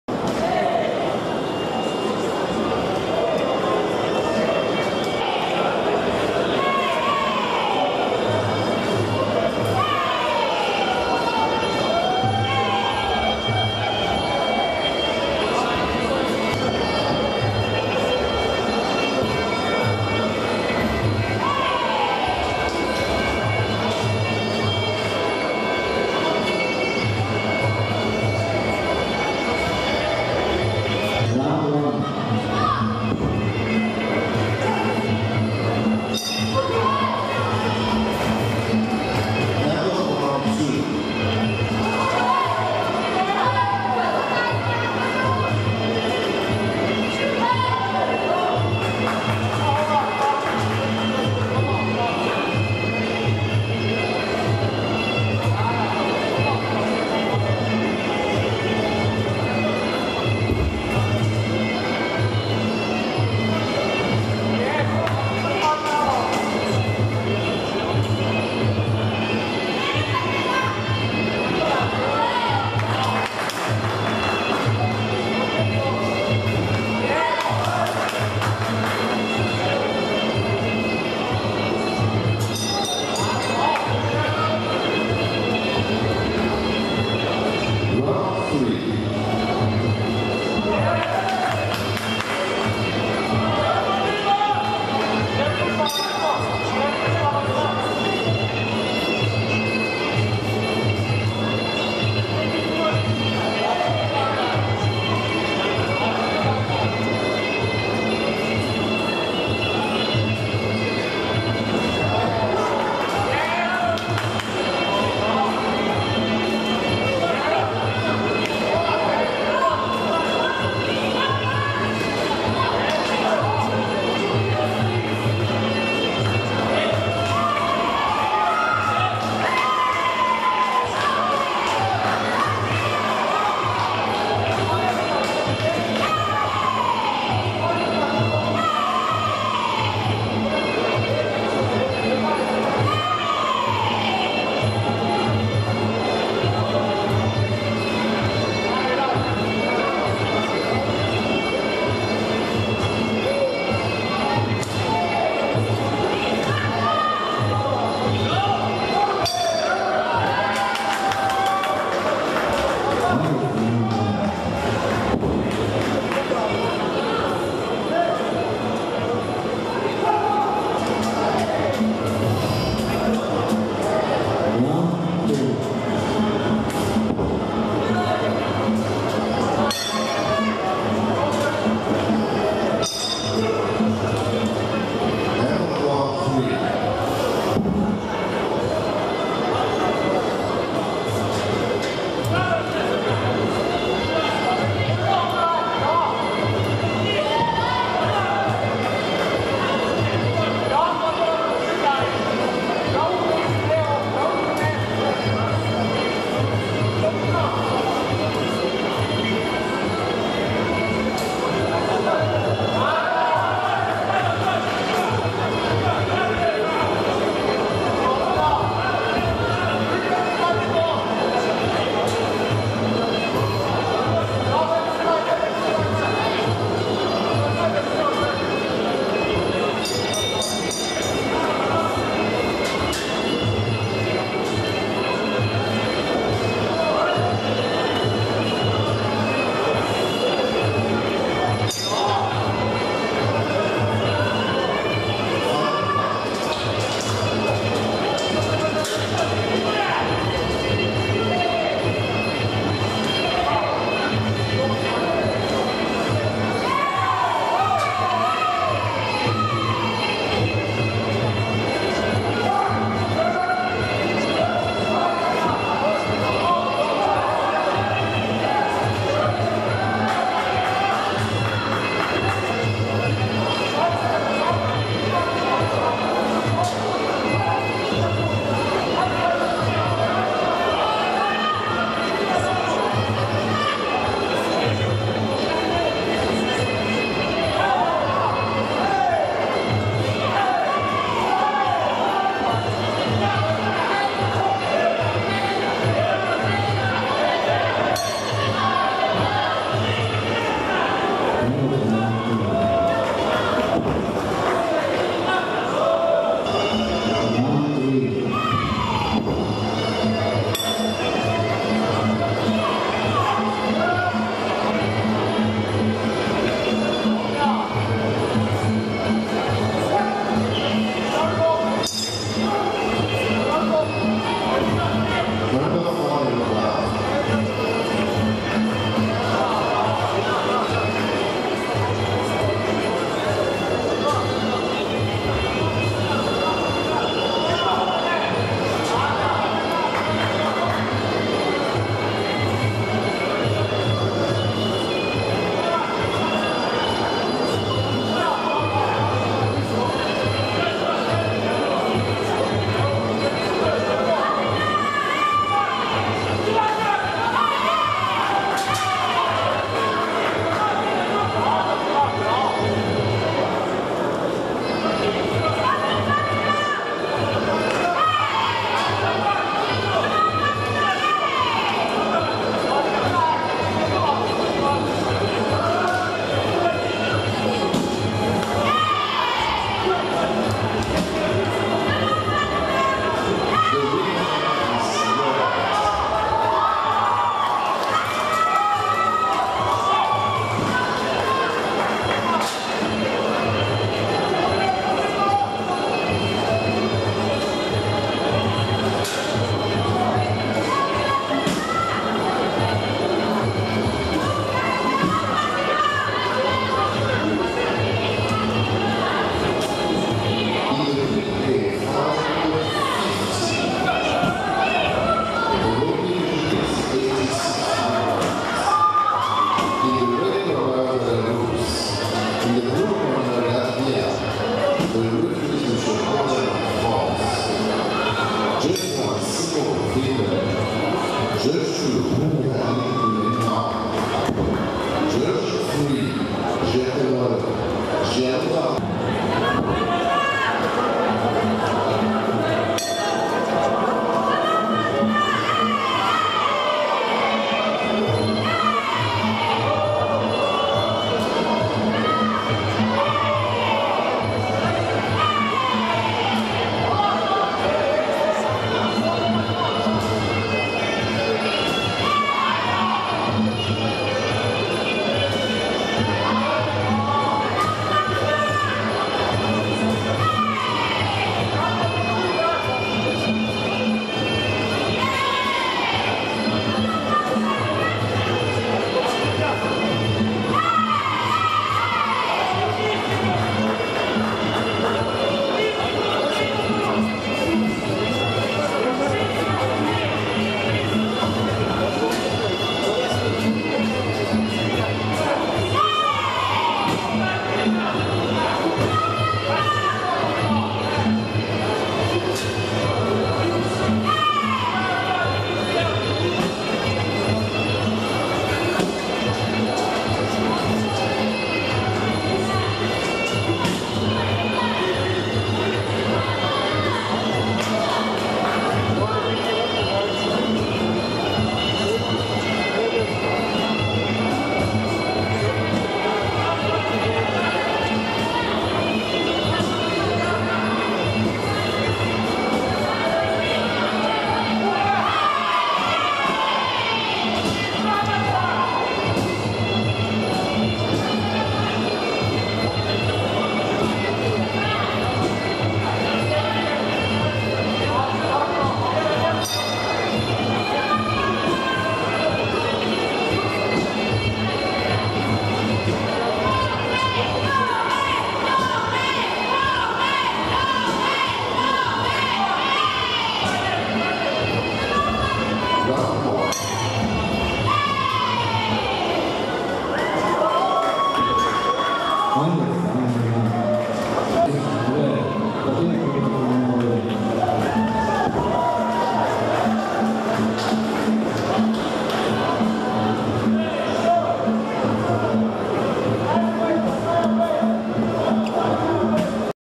Mikhailyuk Tatiana (Ukraine) - Fatima (Norway), female, 51 kg
European Championship of Muay Thai
2011, Antalya, Turkey
Mikhailyuk Tatiana (Ukraine) - Ozdem San (Turkey), female, 51 kg